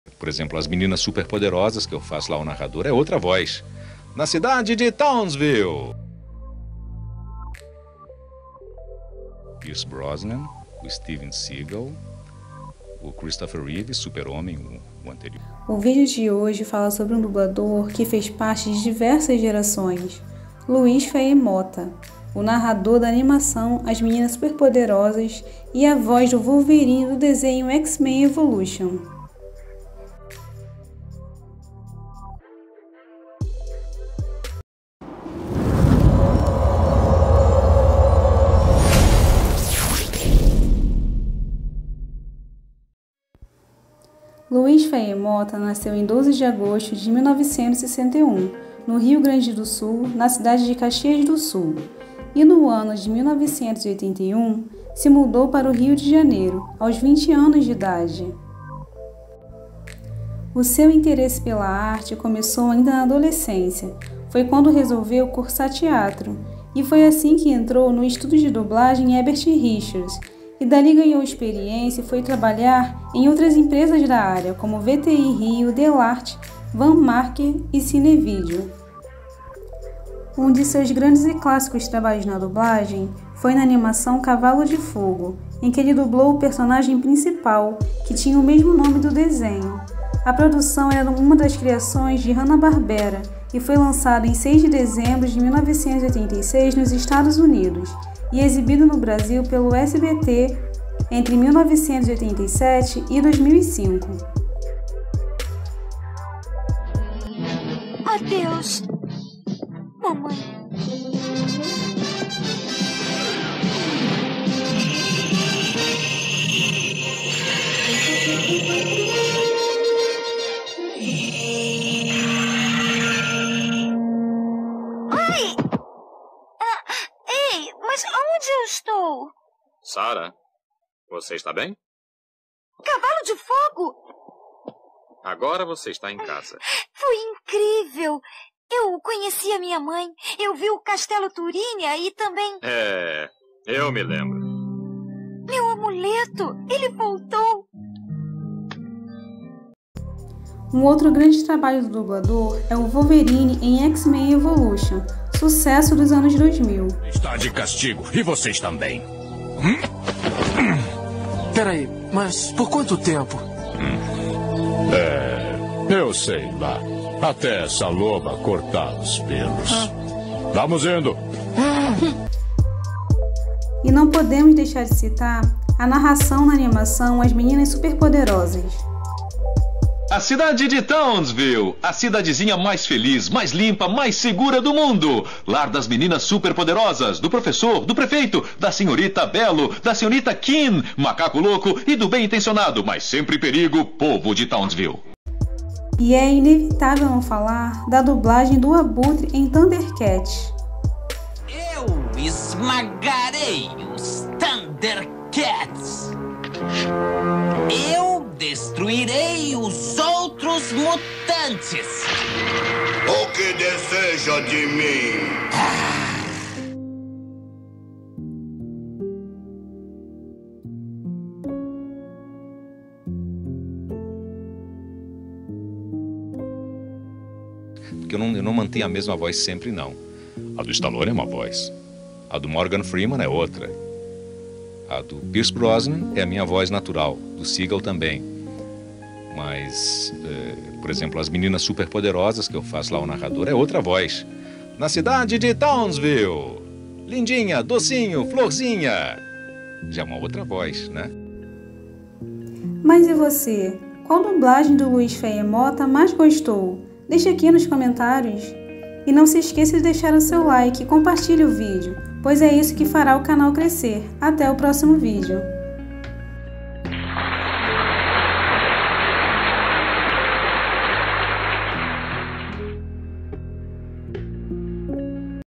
A VOZ DO WOLVERINE E NARRADOR DE AS MENINAS SUPER PODEROSAS | LUIZ FEIER MOTTA DUBLADOR
O dublador Luiz Feier Motta é marcado por dar voz ao Wolverine em X Men Evolution e por narrar a animação As Meninas Super Poderosas, aquela famosa frase: "Na cidade de Townsville". Além de dublar o Cavalo de Fogo e Abutre de Thundercats., ele também dublou o Superman, substituindo André Filho.